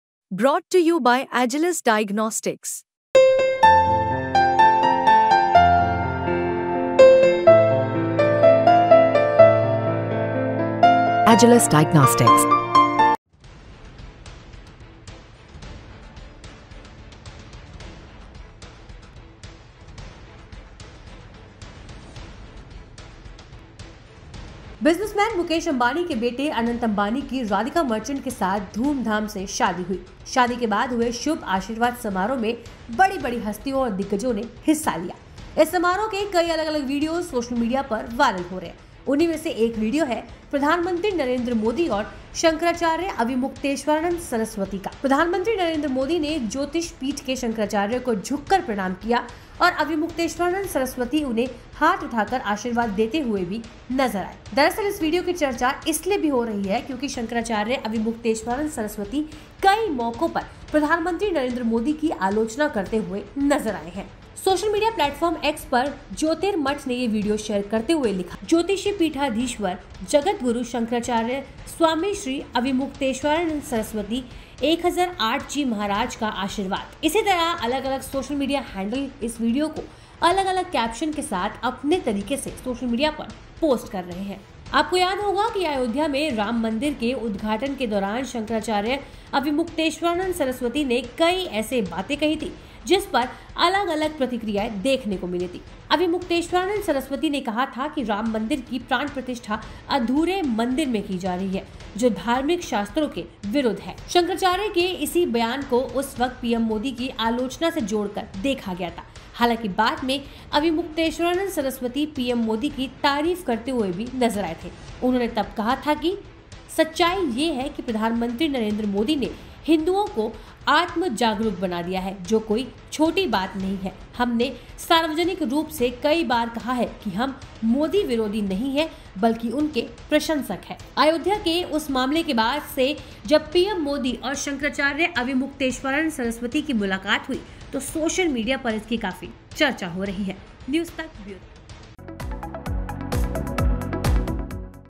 Ambani की शादी में जब शंकराचार्य से मिले PM Modi, देखिए क्या हुआ? Video Viral
उद्योगपति मुकेश अंबानी के बेटे अनंत अंबानी की राधिका मर्चेंट के साथ धूमधाम से शादी हुई... शादी के बाद हुए 'शुभ आशीर्वाद' समारोह में बड़ी-बड़ी हस्तियों और दिग्गजों ने हिस्सा लिया... इस समारोह के अलग-अलग वीडियो सोशल मीडिया पर वायरल हो रहे हैं... उन्हीं में से एक वीडियो है पीएम मोदी और शंकराचार्य अविमुक्तेश्वरानंद सरस्वती का... पीएम मोदी ने ज्योतिष पीठ के शंकराचार्य को झुककर प्रणाम किया और अविमुक्तेश्वरानंद सरस्वती उन्हें हाथ उठाकर आशीर्वाद देते हुए नजर आए थे...